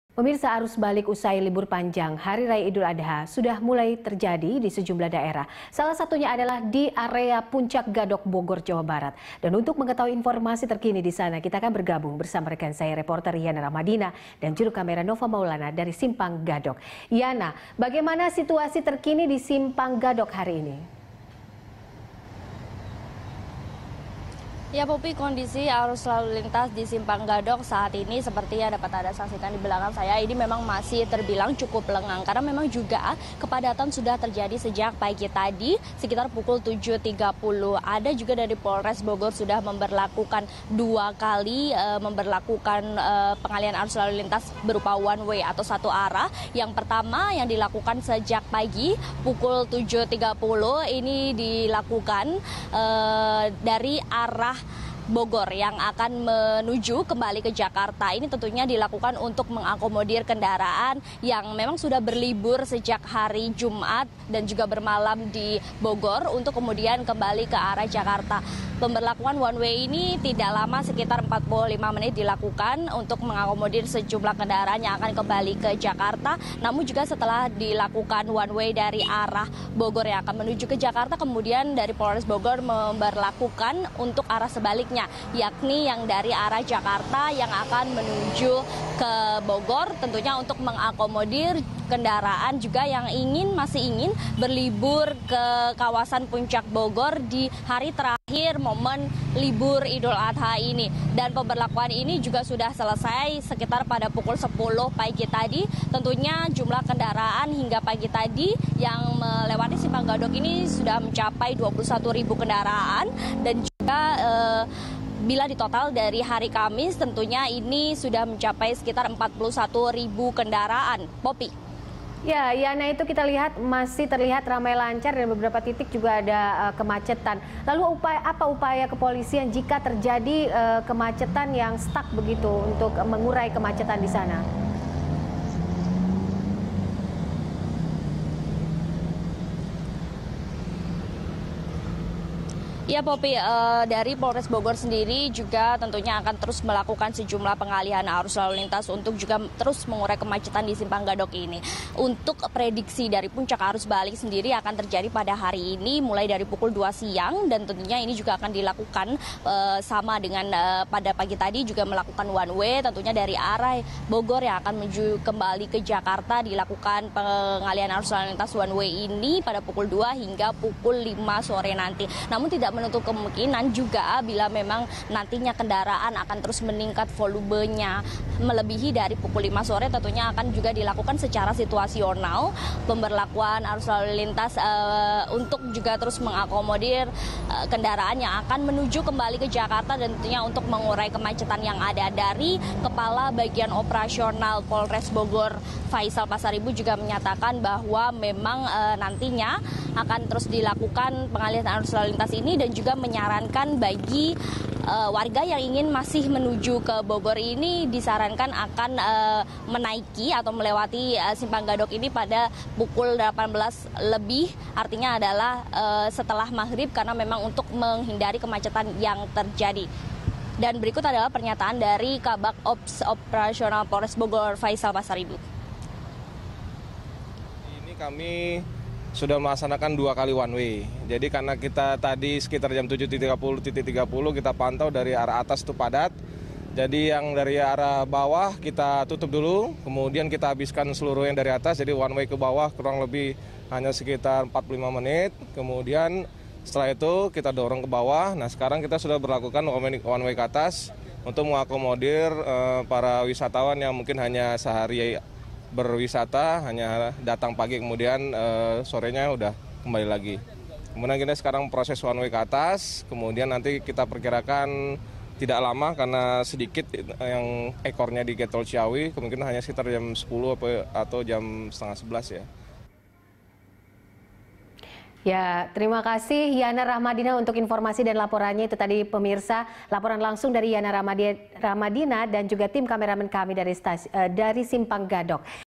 2 Kali Diberlakukan Sistem Satu Arah di Puncak
Arus balik usai libur panjang Hari Raya Iduladha mulai terjadi di sejumlah daerah. Salah satunya di kawasan Puncak, Gadog, Bogor, Jawa Barat.

Facebook.com/BeritaSatuTV
Youtube.com/BeritaSatu